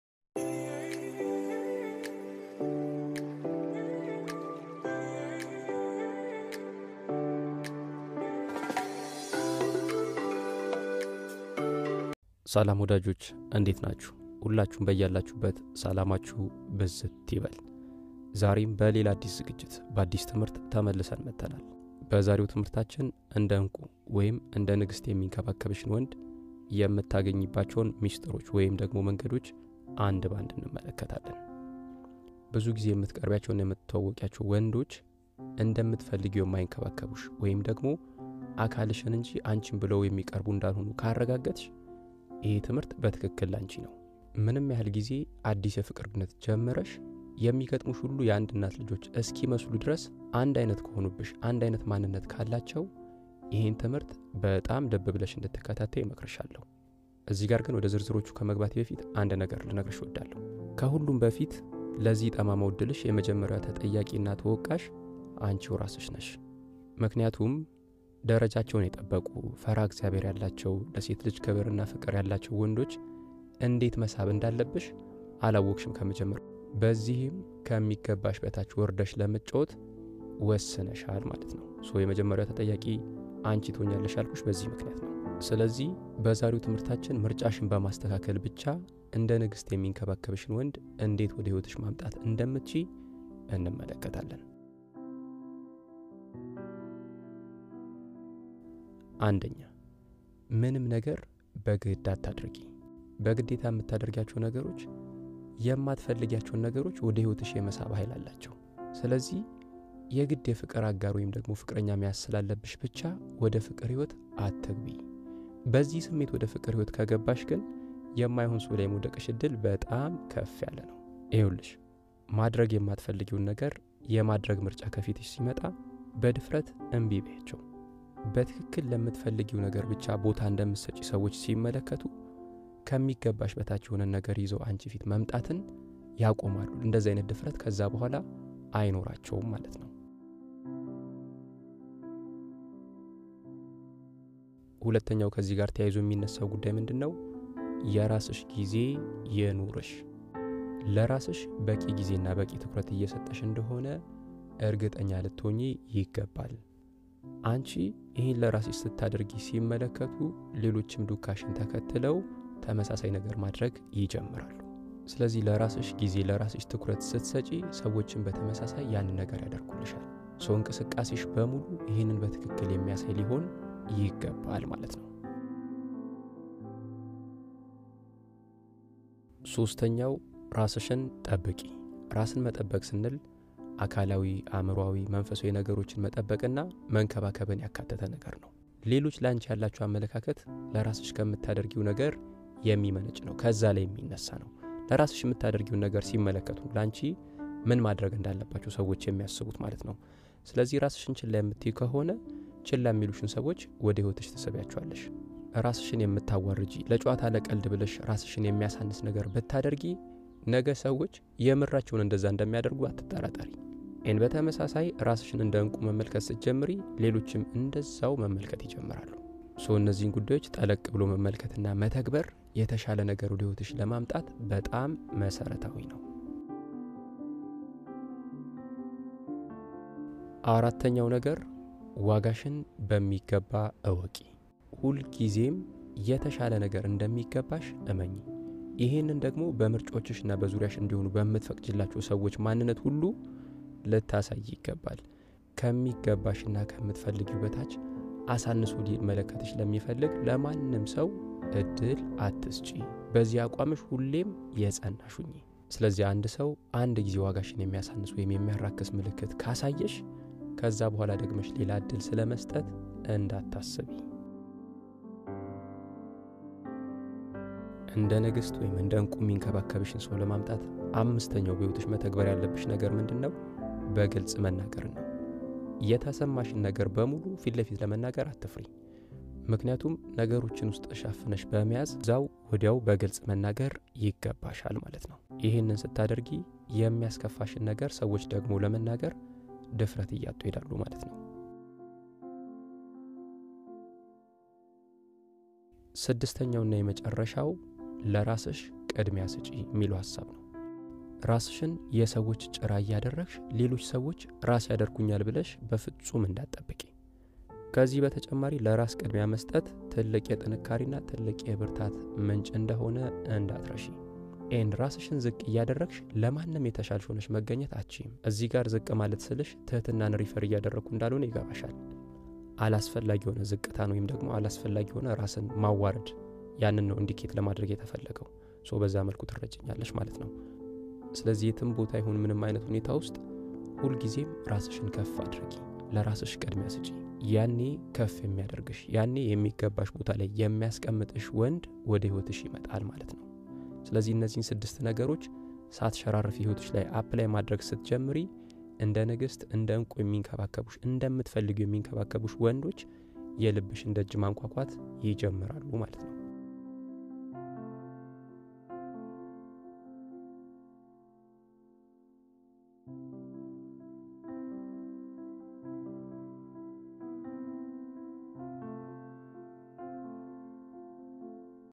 ሁሌም እንደ ንግስት የሚንከባከቡሽንና የሚጠብቁሽን ወንዶች ለመሳብ የሚረዱ ሚስጥሮች | yemefthe bet | Relationship advice for women
Relationship advice for women, make him treat like Queen, he will treat you like gold, be his Queen